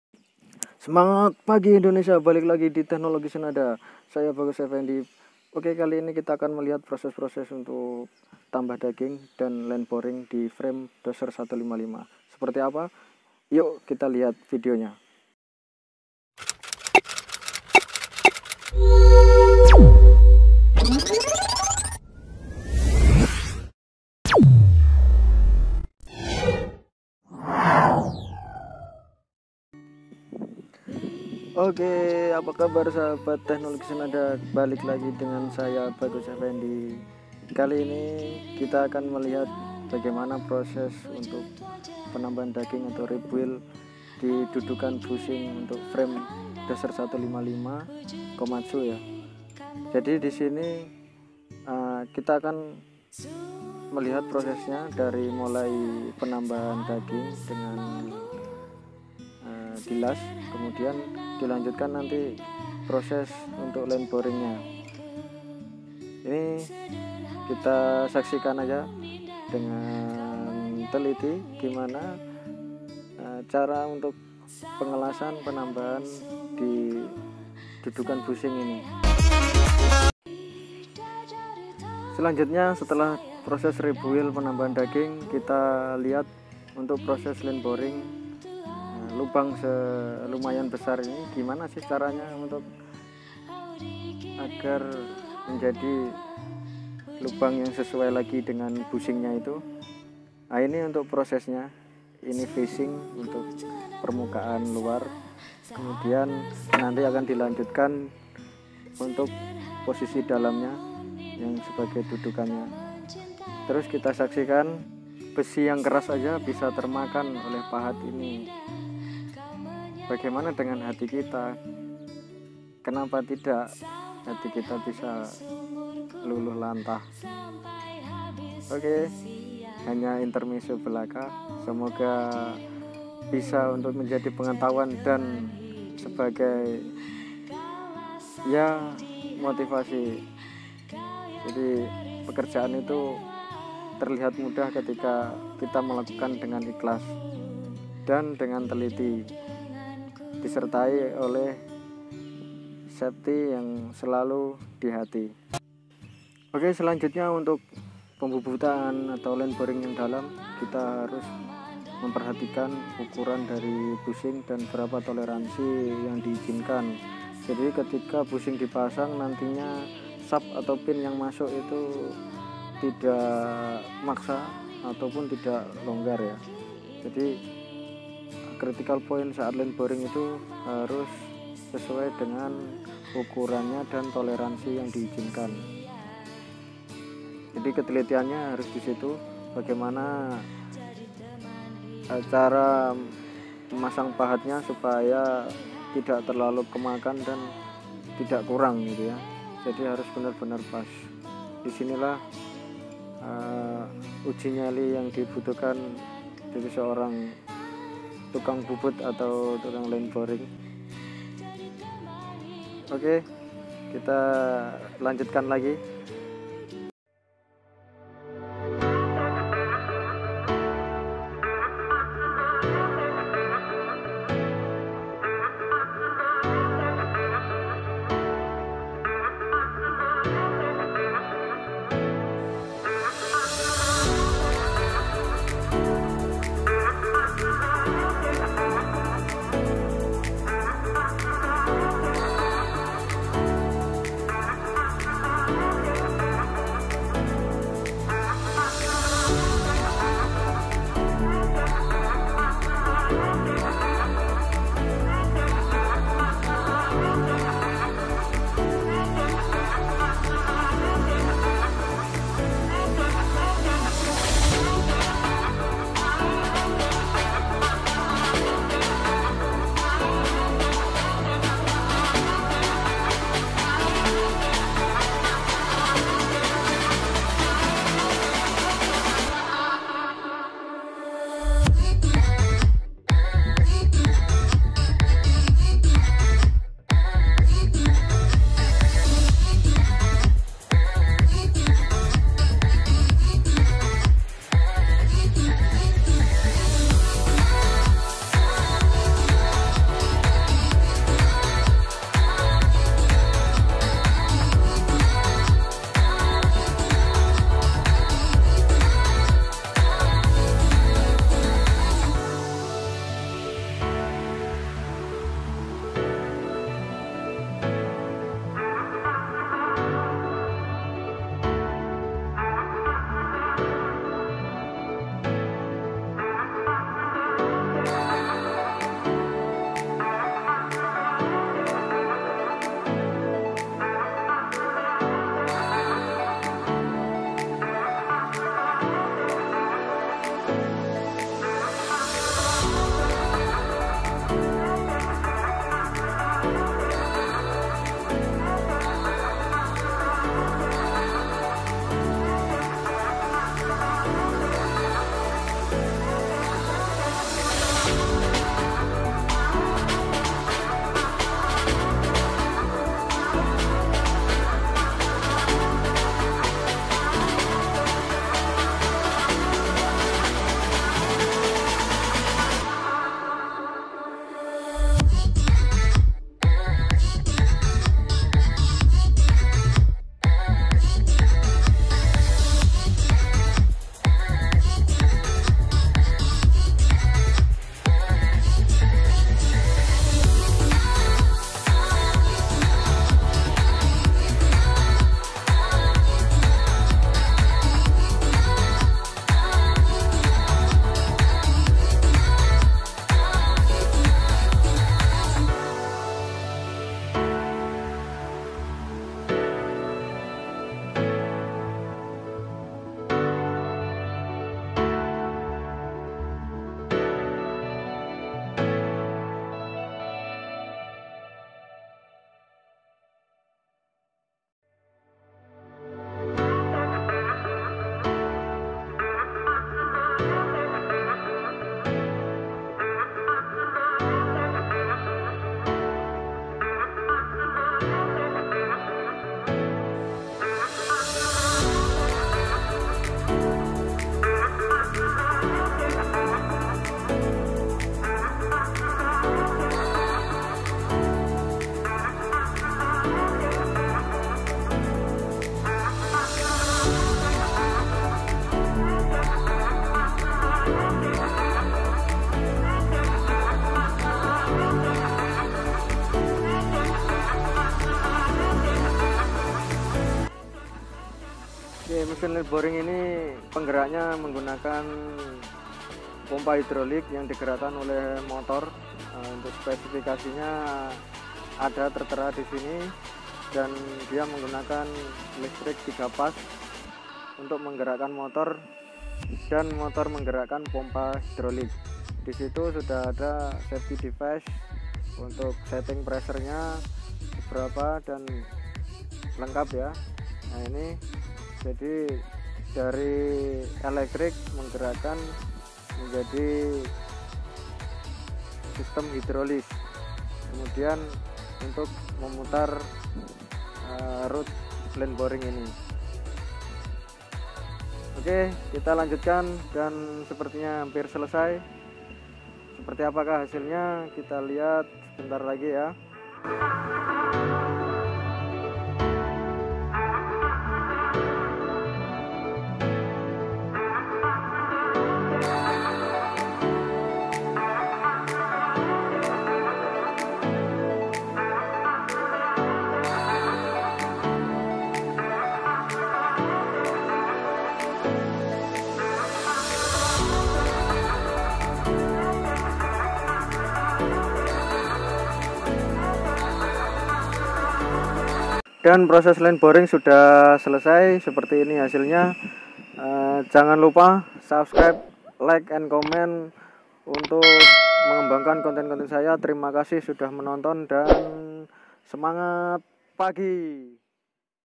Process Line Boring frame Dozer 155
Proses Line Boring frame Dozer 155

Line Boring merupakan proses dimana pemulihan lubang pin atau bising dengan cara tambah daging.